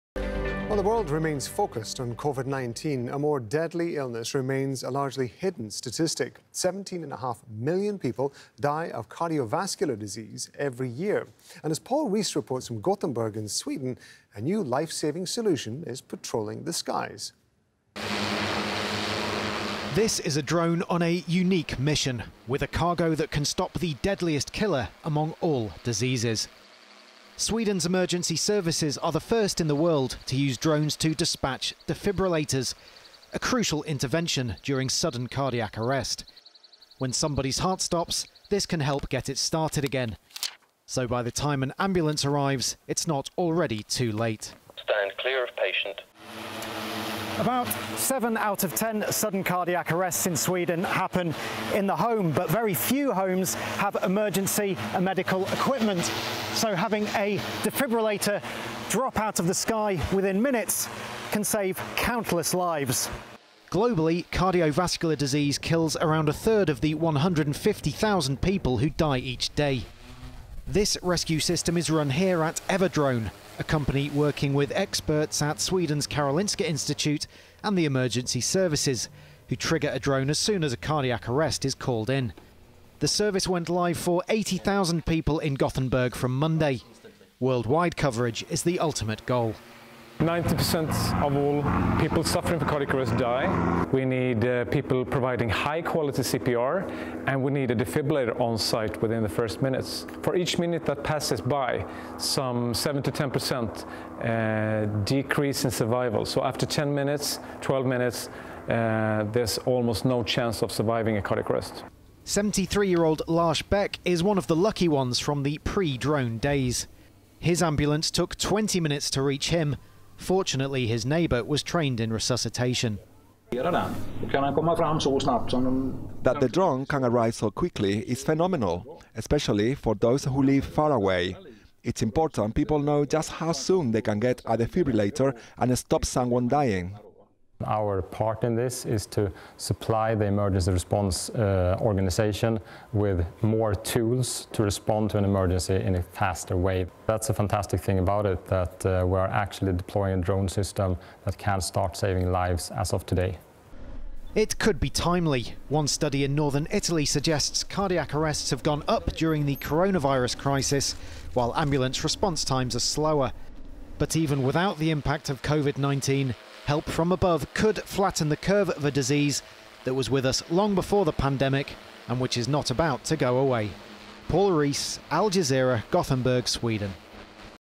Sweden's defibrillator drones, Al Jazeera, 3 June 2020
The operations are part of Everdrone's collaboration with the Centre for Resuscitation Science at Karolinska Institutet and SOS Alarm.